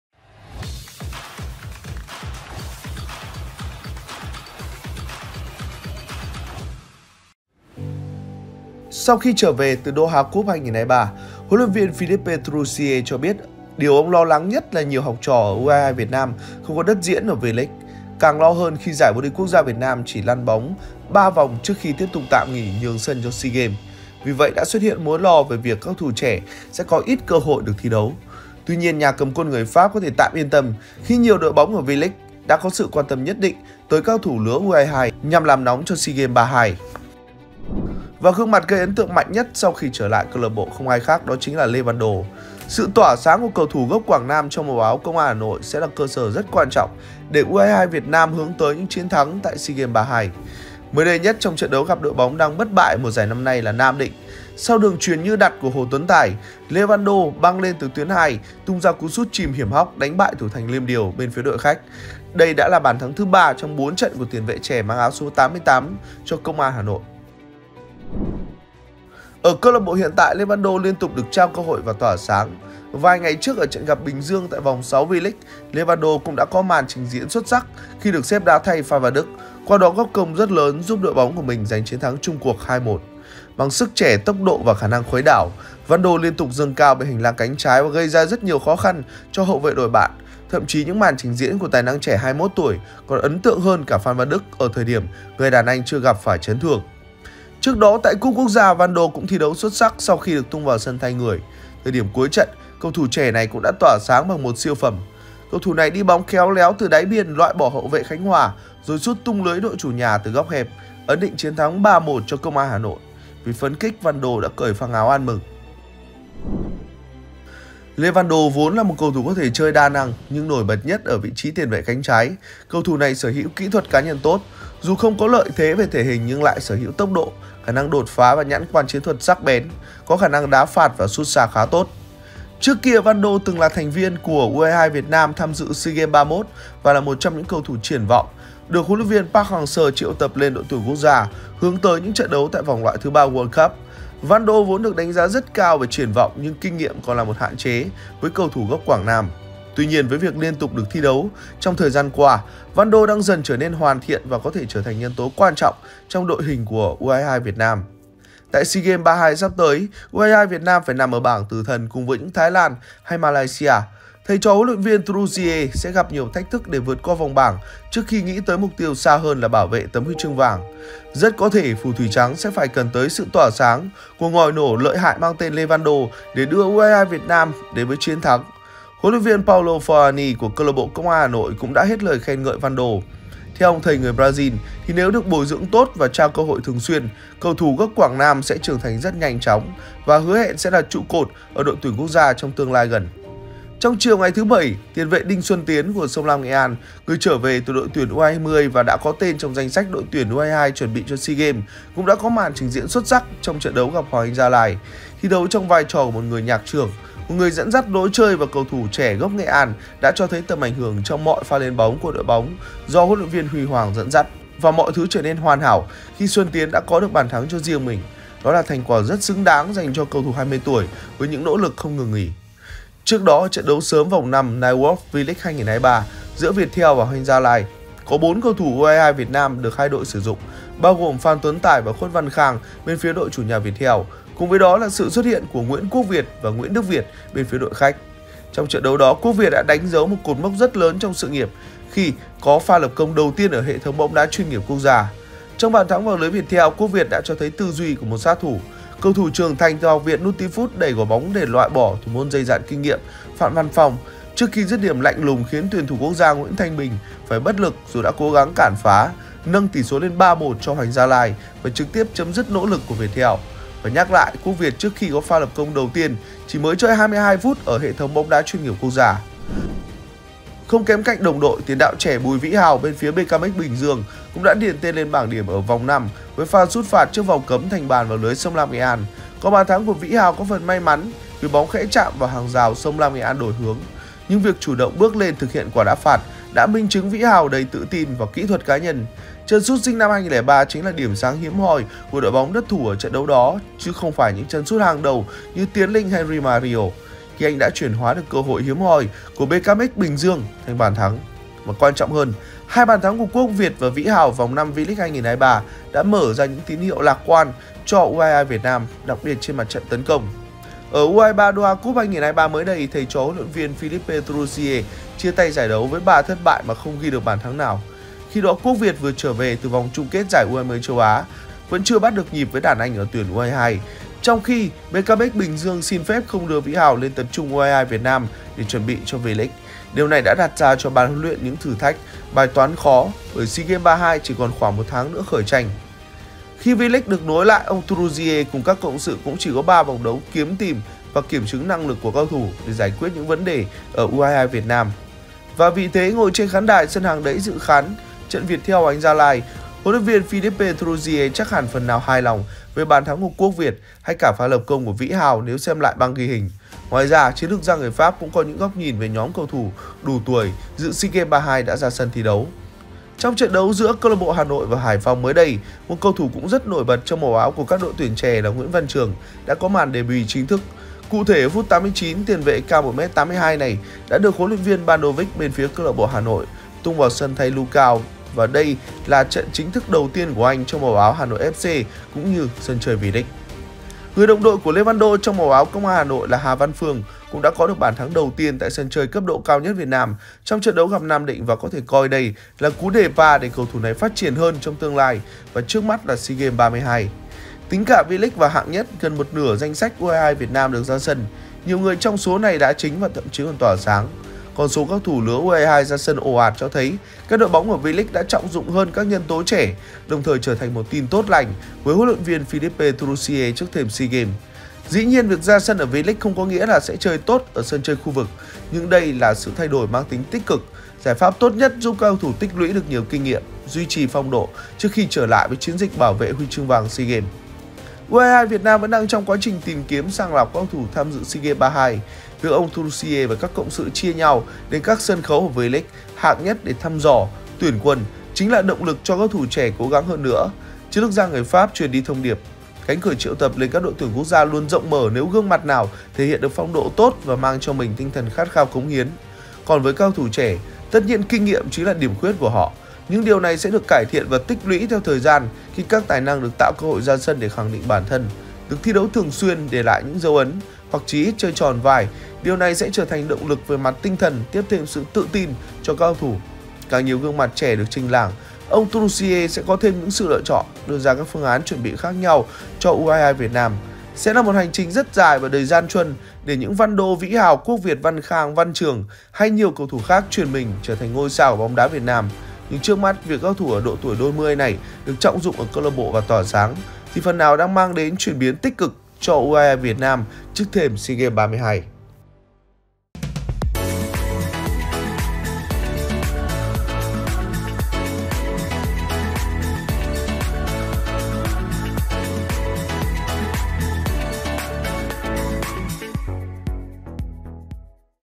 SỰ BÙNG CHÁY CỦA CÁC CẦU THỦ U23 KHI TRỞ VỀ CLB
Sẽ là một hành trình rất dài và đầy gian truân để những Văn Đô, Vĩ Hào, Quốc Việt, Văn Khang, Văn Trường hay nhiều cầu thủ khác chuyển mình trở thành ngôi sao của bóng đá Việt Nam. Nhưng trước mắt, việc các cầu thủ ở độ tuổi đôi mươi này được trọng dụng ở CLB và tỏa sáng, thì phần nào đang mang đến chuyển biến tích cực cho U22 Việt Nam trước thềm SEA Games 32.